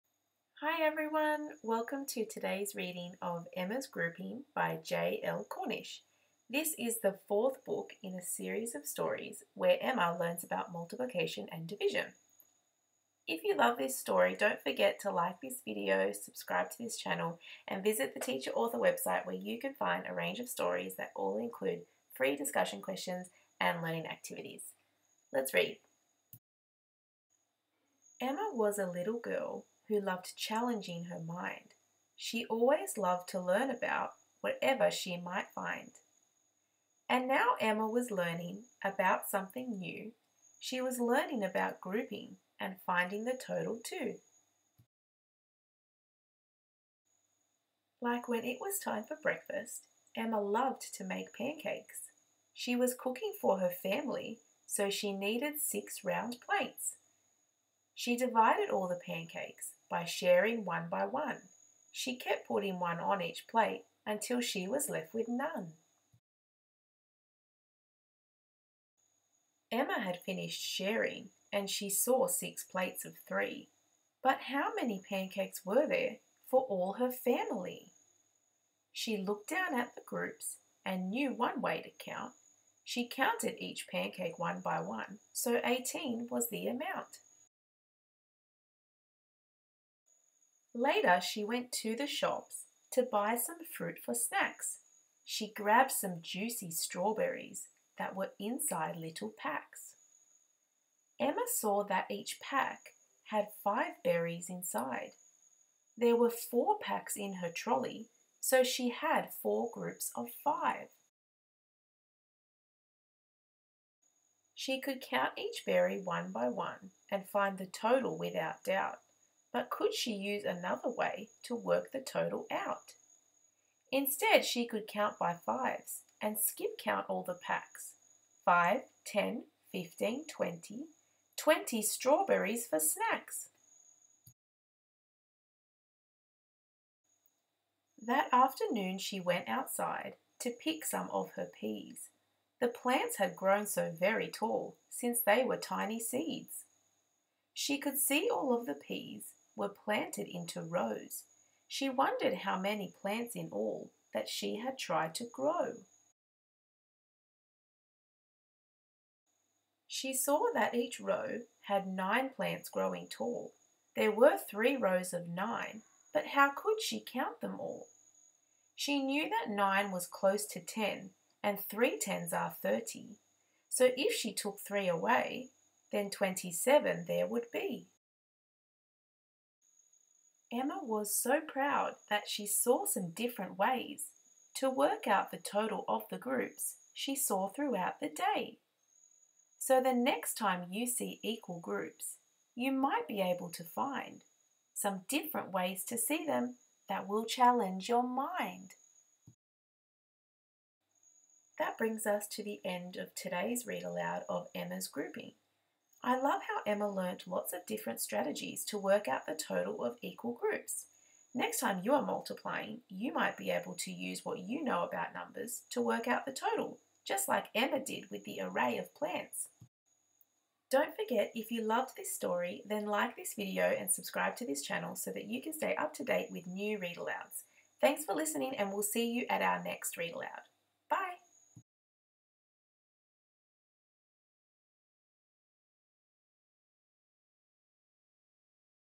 Emma's Grouping | JL Cornish | Readaloud | Multiplication Book | Picture Book | Multiplicative
Emma loves to learn about groups that she sees around her. She sees equal groups when sharing pancakes with her family, buying snacks at the shops and even when picking plants in her garden!

Join Emma as she learns some flexible ways to find the total of equal groups. Suitable for children aged 4-10.

for more educational stories.
Follow @the.teacher.author on Instagram and Facebook for lesson ideas and more!